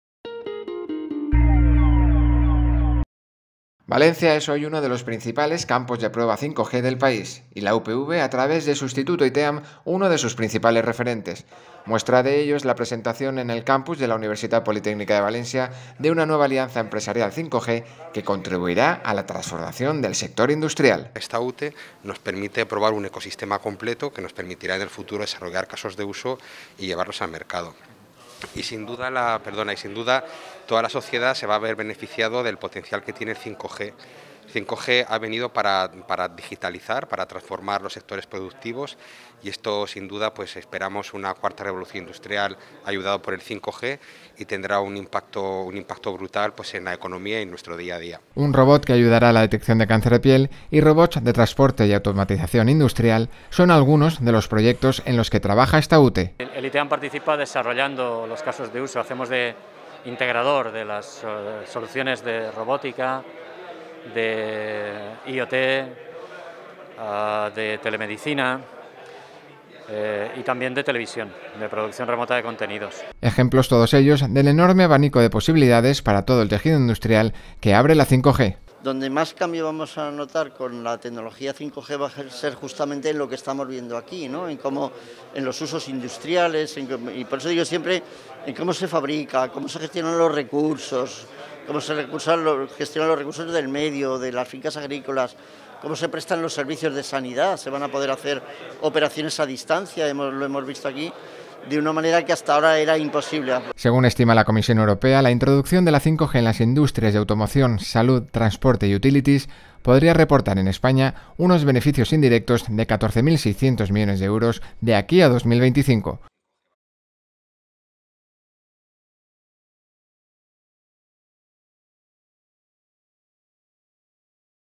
Presentación UTE 5G - Noticia @UPVTV, 16-06-2021
La Universitat Politècnica de València (UPV) acogió la presentación de la Unión Temporal de Empresas (UTE) sobre 5G formada por Orange, Visyon (Grupo MEDIAPRO), Robotnik, CFZ Cobots, Elewit (Grupo Red Eléctrica), Aracnocoptero, Idrica y ETRA. La UTE trabaja en el desarrollo de nuevas aplicaciones y servicios 5G que permitirán impulsar la transformación digital de la sociedad y el tejido empresarial en España. Cuenta con la colaboración de diferentes entidades, entre ellas la Universitat Politècnica de València -a través del Instituto iTEAM-, el Ayuntamiento de València y la Generalitat Valenciana.